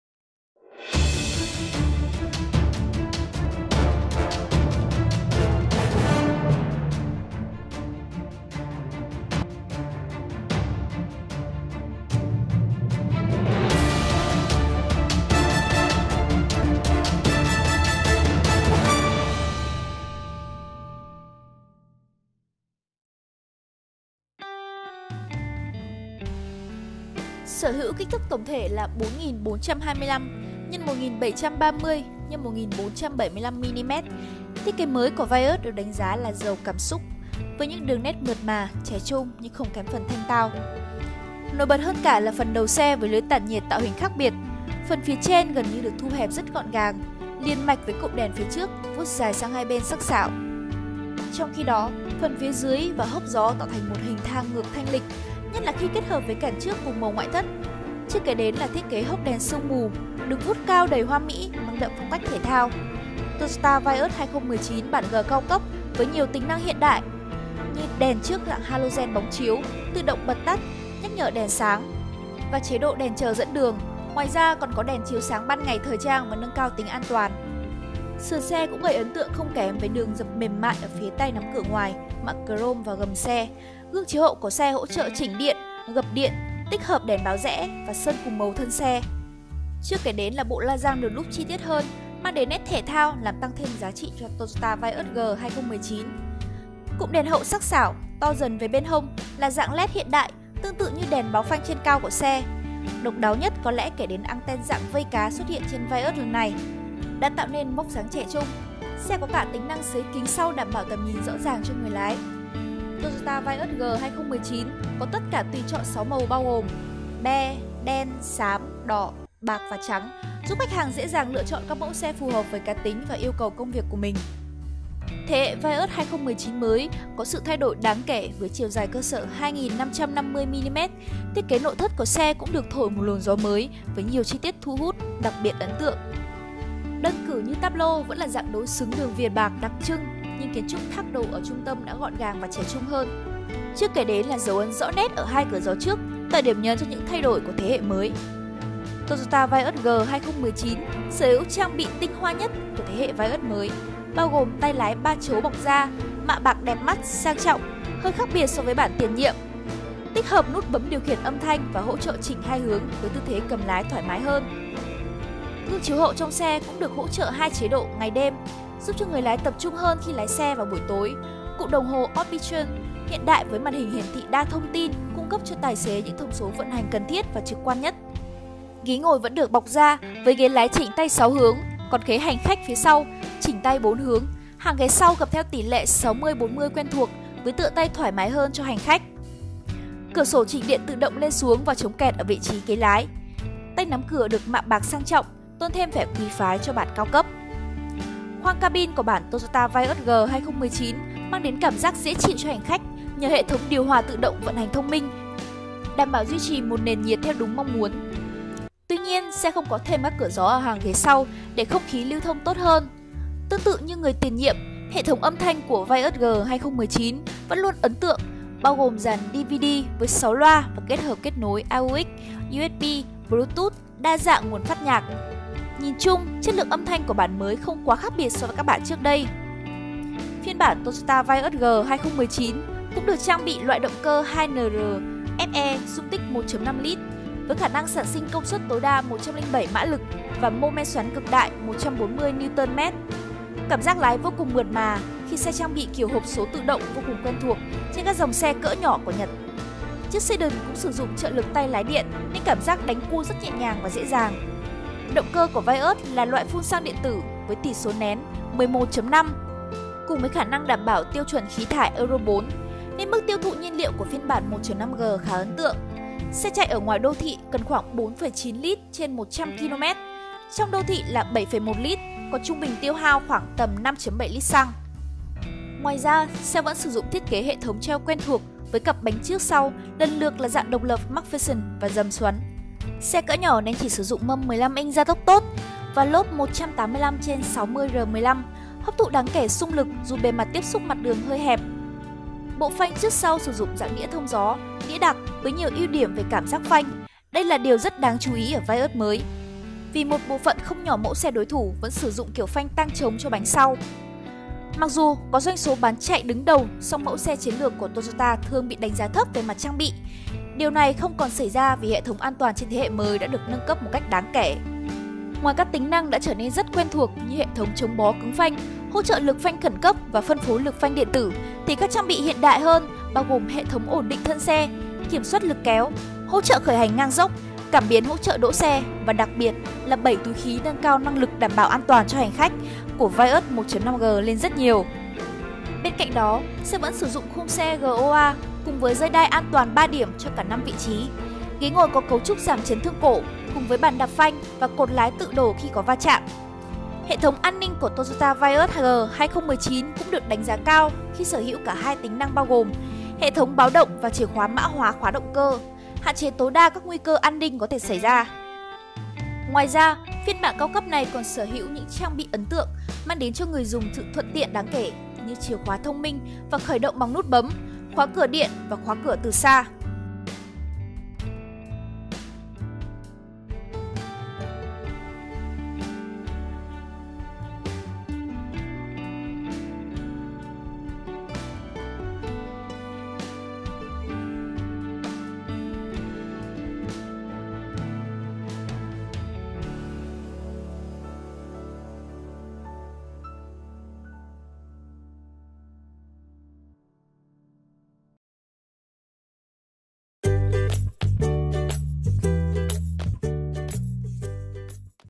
Toyota Vios G 2020 - Giá chỉ 570 triệu, ngang giá Accent Full, tội gì chọn xe Hàn?
Giá xe Toyota mới nhất Giá xe Hyundai cập nhật mới nhất phiên bản MT base, MT full, AT thường, AT full.
Hyundai Lê Văn Lương - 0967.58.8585

Chi tiết khuyến mại Xem chi tiết tại Hyundai Vios 2020 khuyến mại lớn.